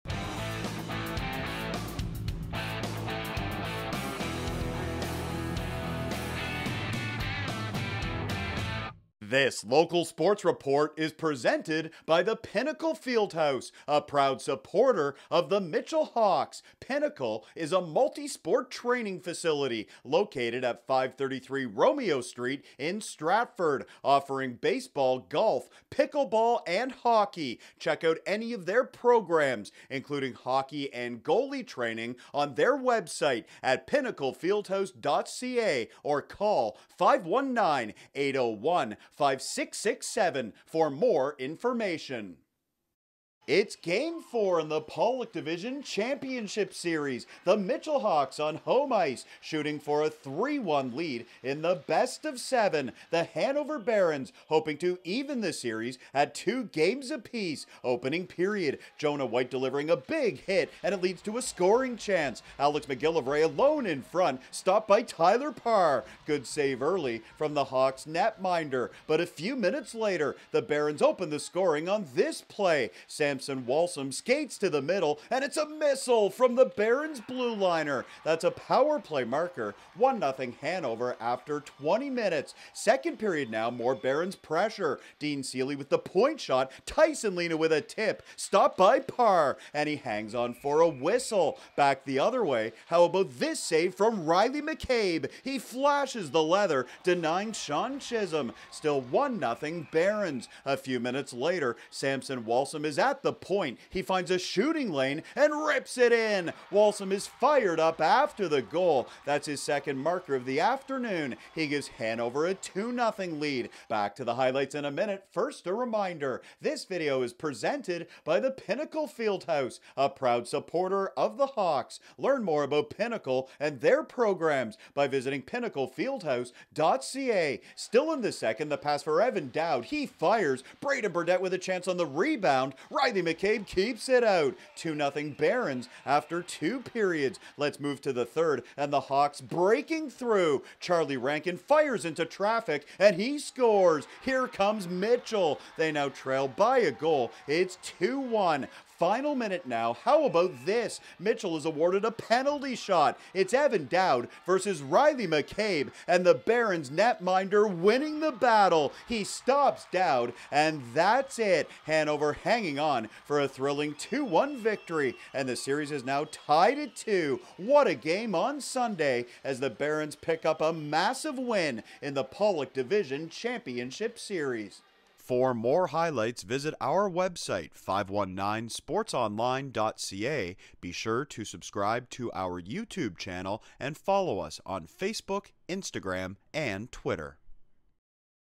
Pollock Division Championship (Game 4) - Hanover Barons vs Mitchell Hawks
Highlights from Sunday April 10th between the Barons and Hawks in the PJHL playoffs.

This video is presented by the Pinnacle Fieldhouse at 533 Romeo Street in Stratford. Learn more about Pinnacle and their programs by visiting